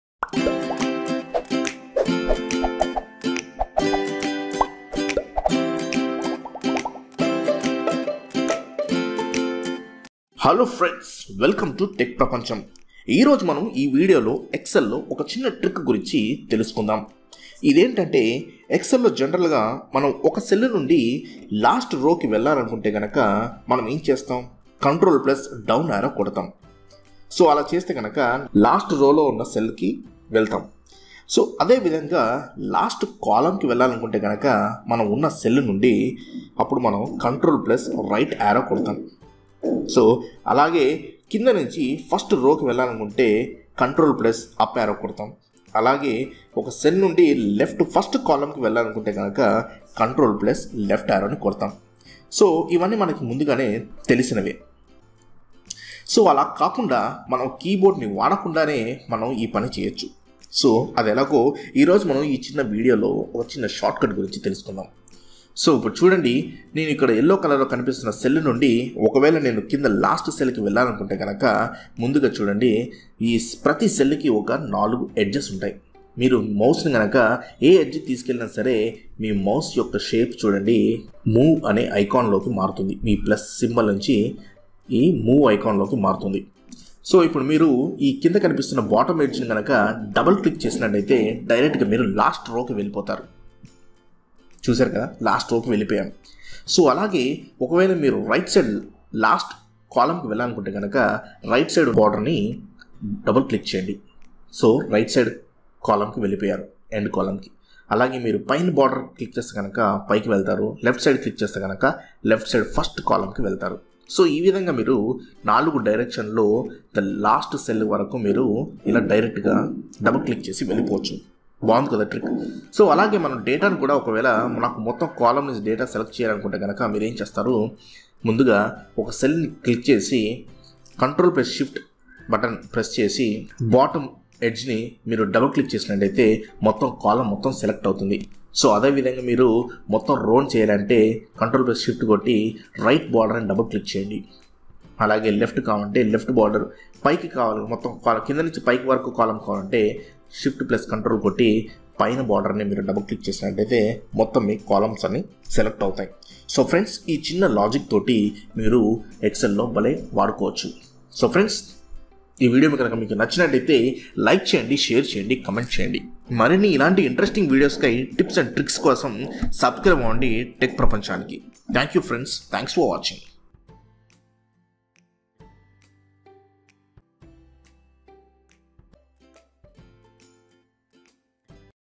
How To Go To Last Row Last Column With Data In Excel | Telugu | Excel Tricks | Techprapancham
How To Go To Last Row Last Column With Data In Excel | Telugu | Excel Tricks And Tips